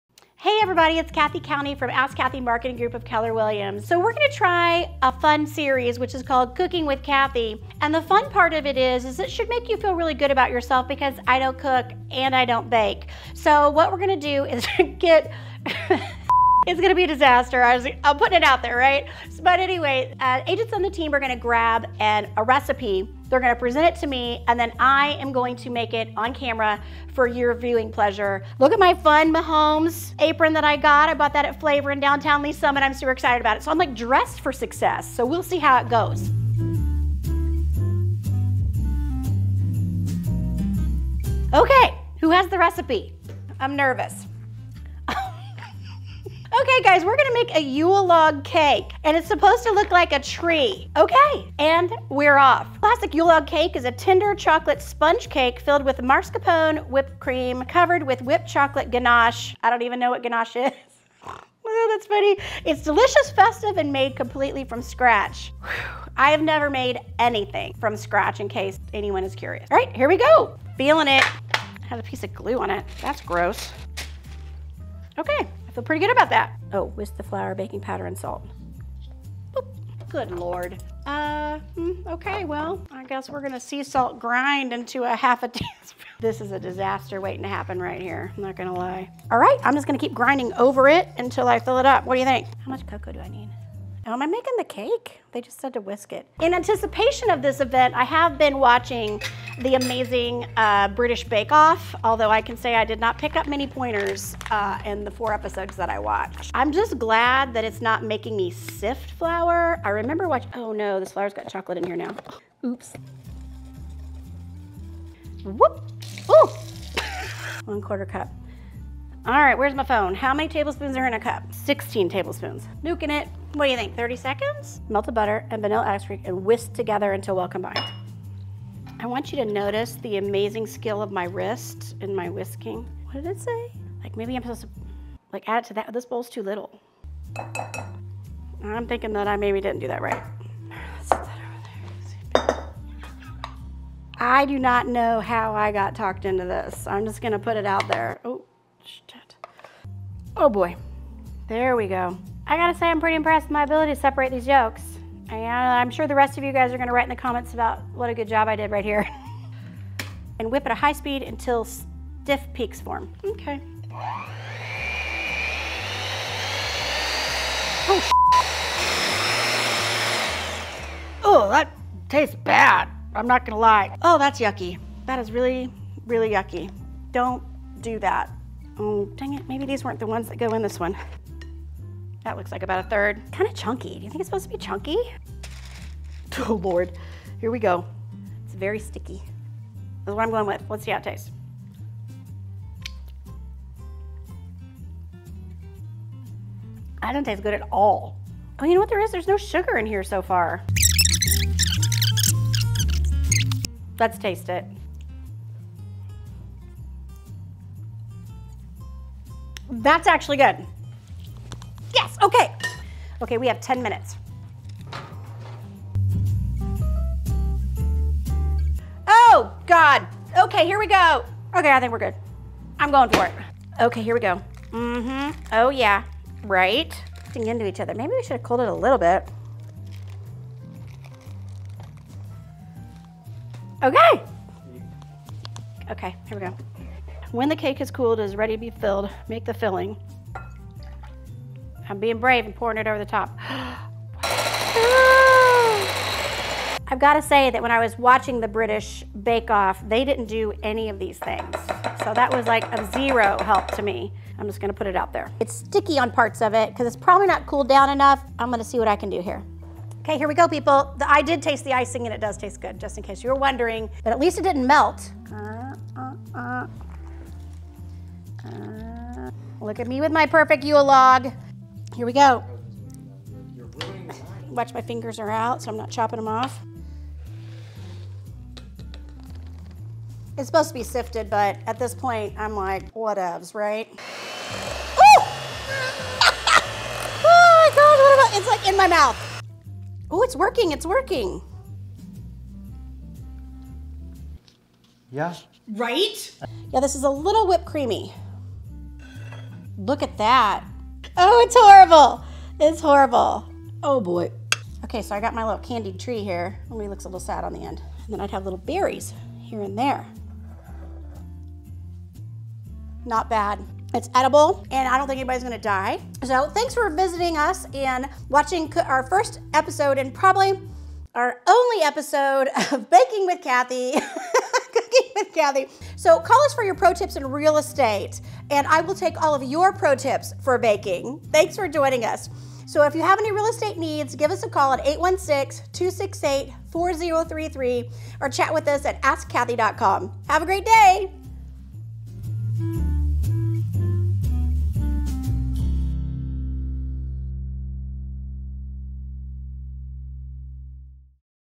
Cooking with Cathy: Holiday Edition!
Our fearless leader is challenged with recreating an EPIC Yule Log Cake!

Looking for the recipe?

Let us know how you did by tagging us in your posts on Facebook @AskCathyRealEstate or on Instagram @AskCathyMarketingGroup. Happy Baking!